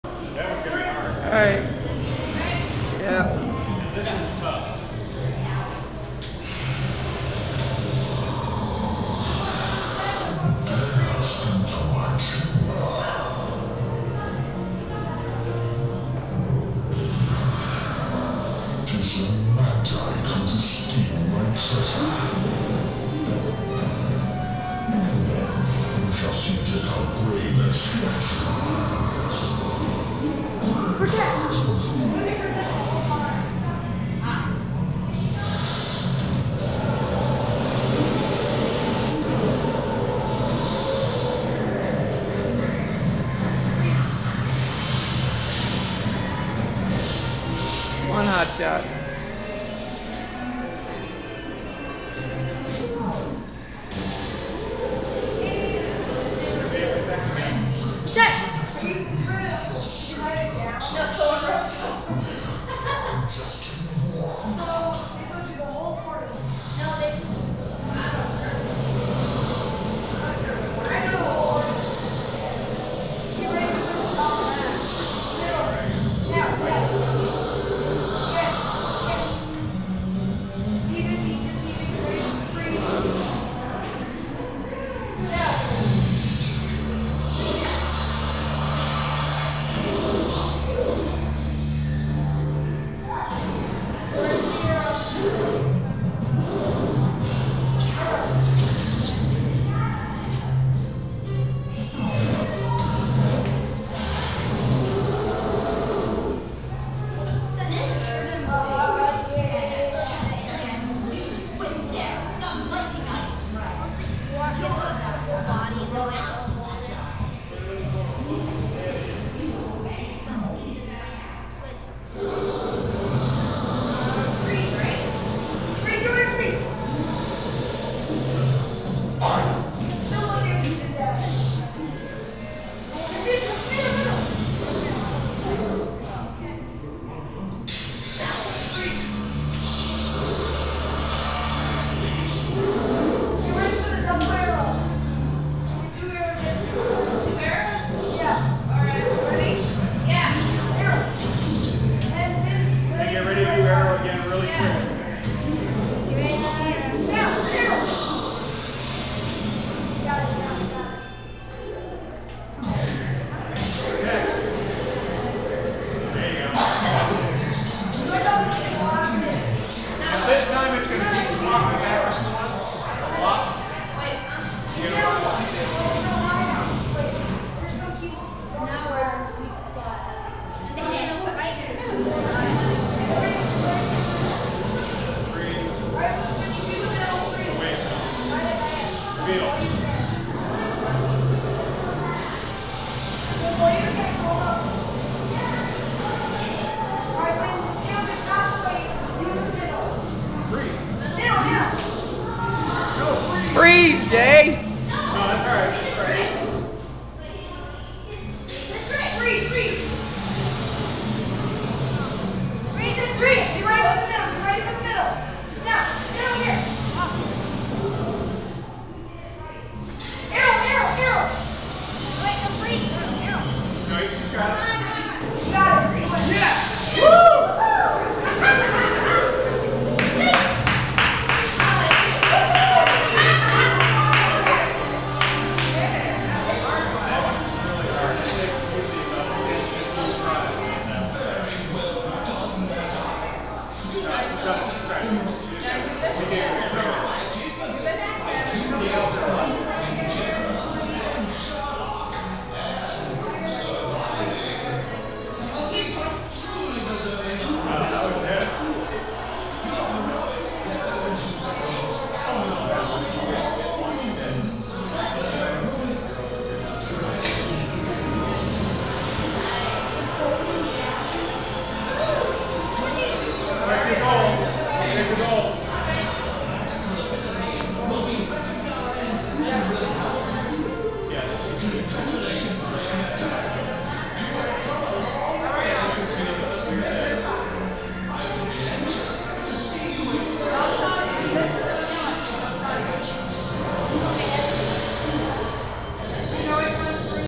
Magiquest: Heroic Dragon Adventure
Myrtle Beach, South Carolina. I am fighting both fire and ice dragons at the same time. It is very hard to do. I got help from  my fellow Magi